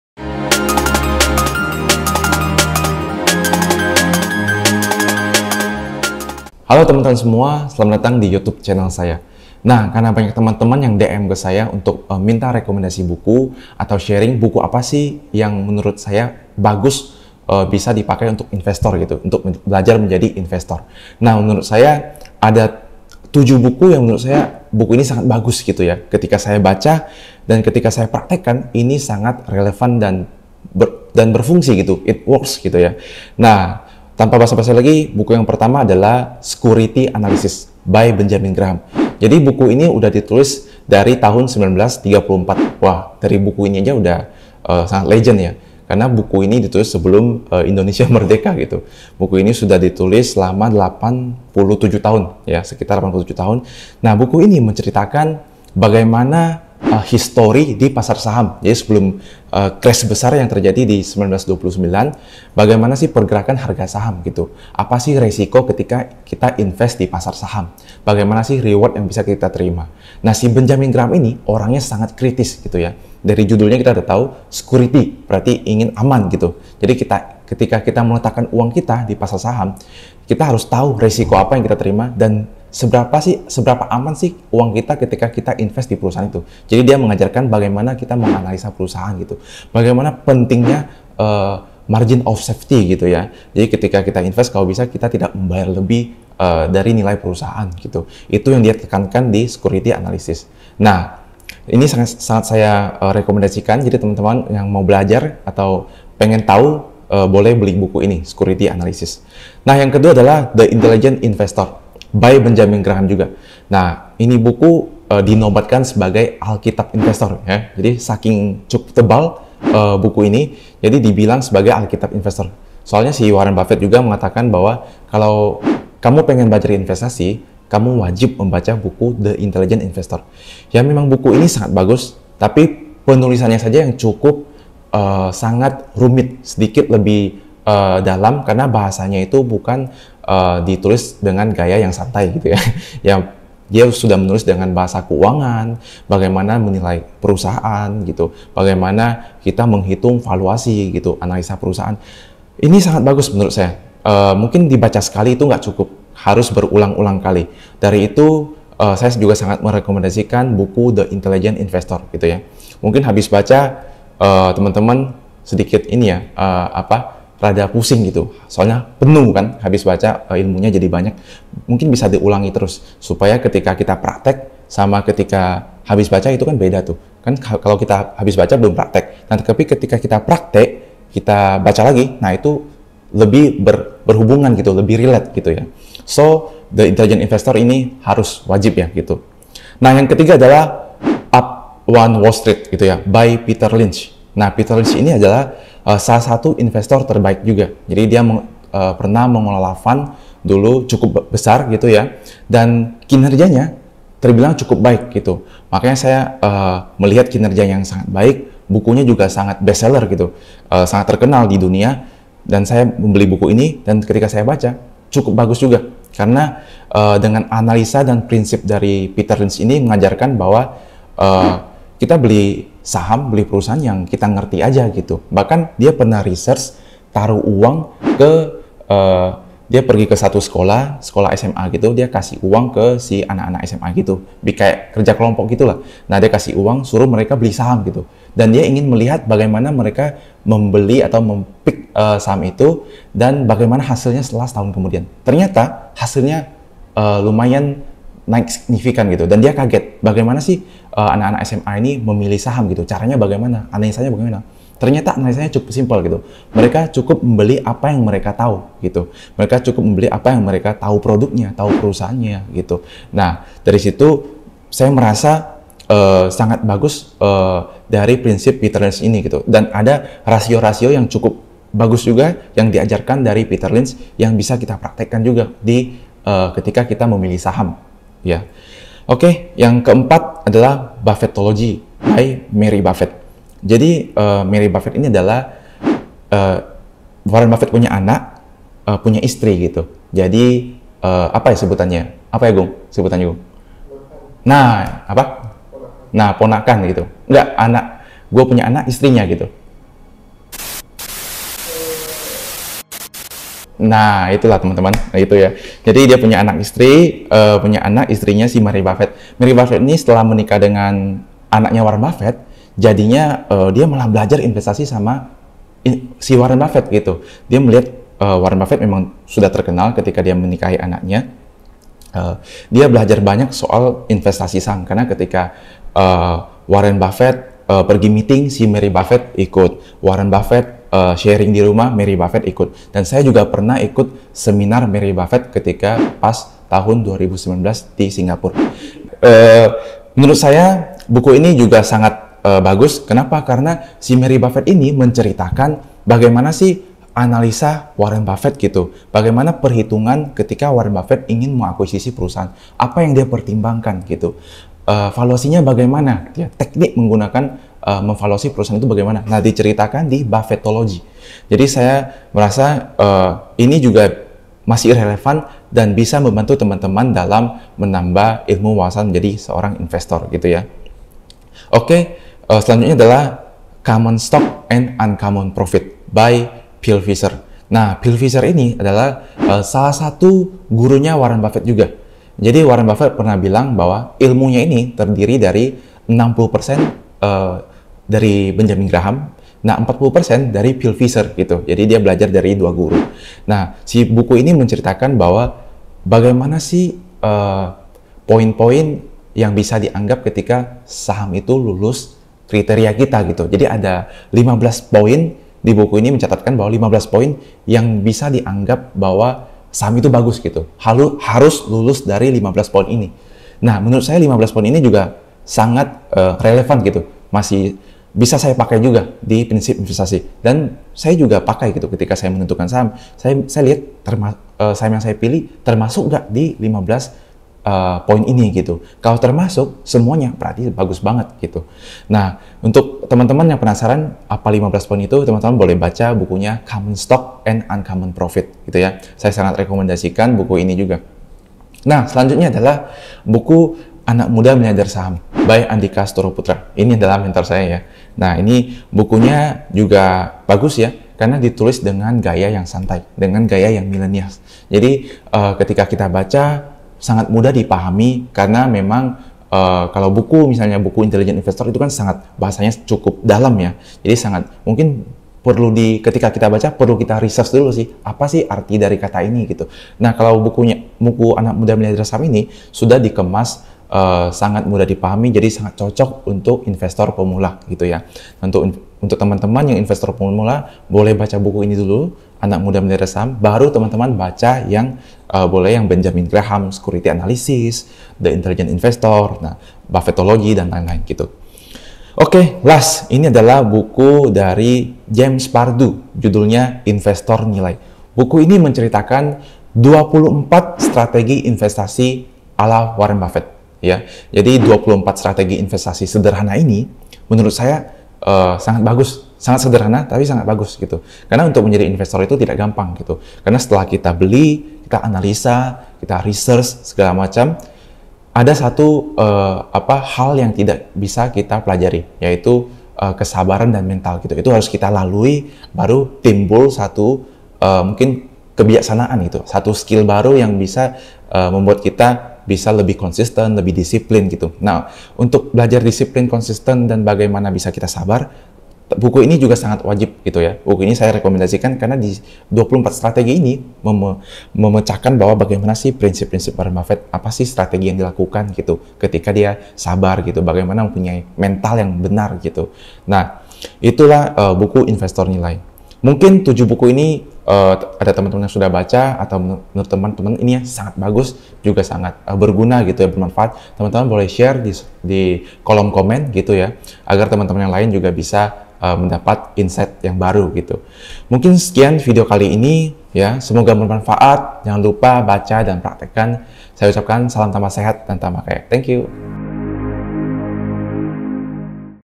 7 Rekomendasi Buku Buat belajar Investasi Saham | Tomy
Ini dia 7 rekomendasi buku untuk belajar investasi saham yang menurut saya wajib dibaca untuk belajar menjadi investor!

00:00 Opening
00:36 Buku Pertama: Security Analysis
01:50 Buku Kedua: The Intelligent Investor
03:10 Buku Ketiga: One Up On Wall Street
04:40 Buku Keempat: Buffetology
06:13 Buku Kelima: Common Stock and Uncommon Profits
07:23 Buku Keenam: Anak Muda Miliarder Saham
08:13 Buku Ketujuh: Investor Nilai

Semoga bermanfaat, salam